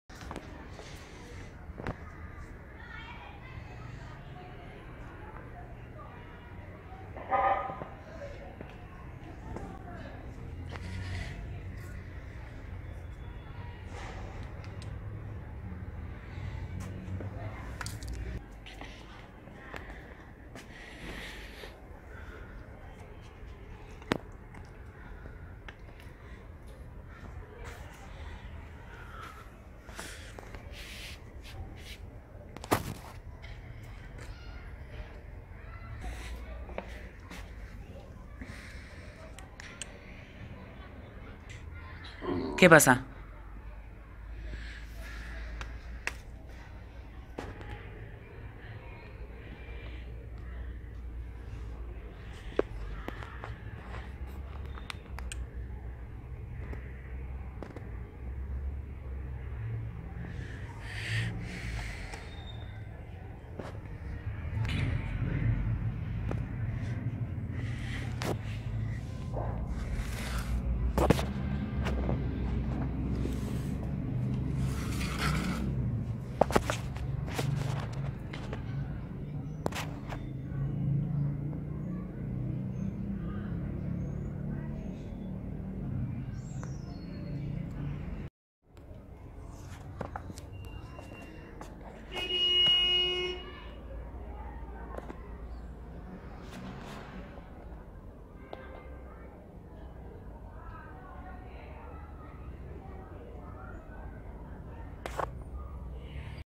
Akita Inu VS Turtle 🐢- Reaction
Bolt meets the franklin tortoise for the first time